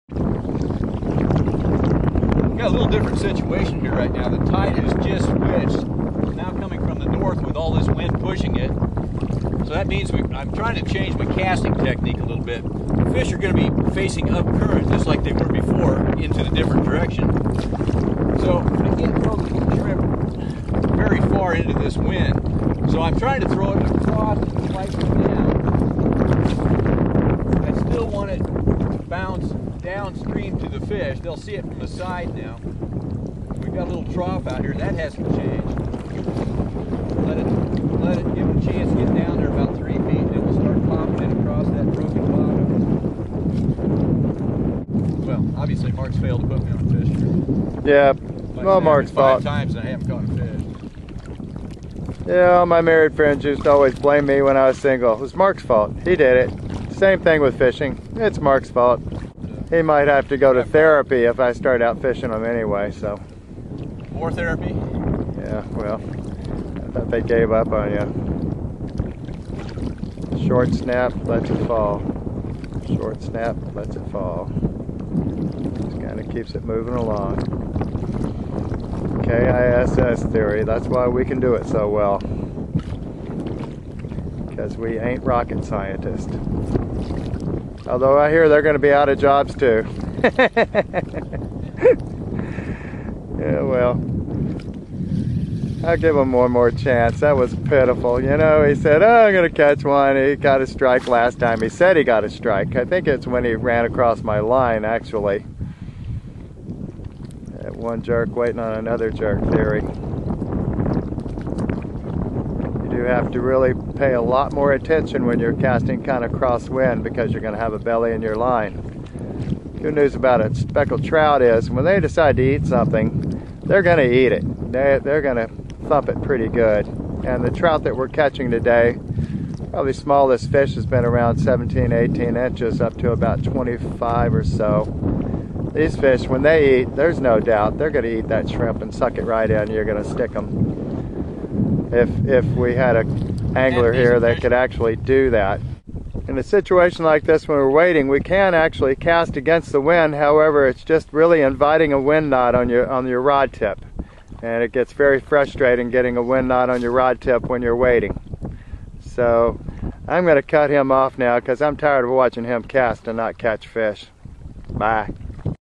DOA Shrimp Casting Technique
Mark and Jerry demonstrate how to fish the DOA Shrimp in windy conditions.